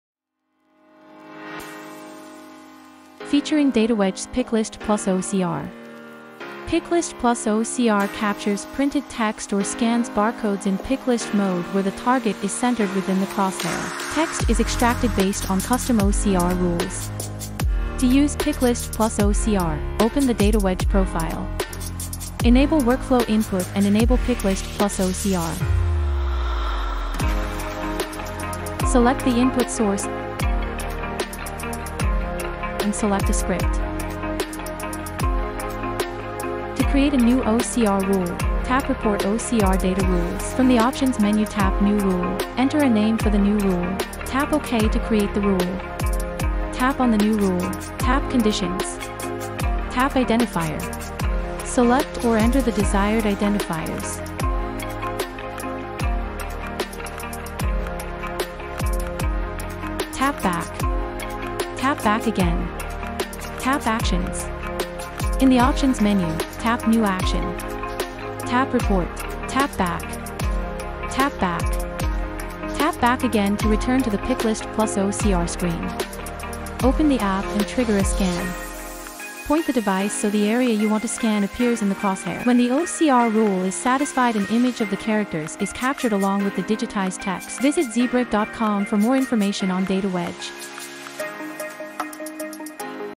How to Use DataWedge's Picklist + OCR | Zebra
Dive into this video tutorial on using DataWedge's Picklist+OCR feature. Learn about DataWedge's Picklist and OCR (optical character recognition) feature to capture a barcode or digitize printed text.  Find out how to capture printed text or scan barcodes in picklist mode, with the target centered within the crosshair. Follow a step-by-step guide to enable 'Workflow Input' and 'Picklist+OCR', select an input source, and create a custom OCR rule. Discover how to create a new rule, choose your identifiers and set up new actions. See how to effectively trigger a scan and capture an image along with the digitized text when the OCR rule is satisfied. This video is a must-watch for anyone looking to enhance their data capture efficiency with DataWedge. To find out more about DataWedge's Picklist and OCR, Learn about OCR technology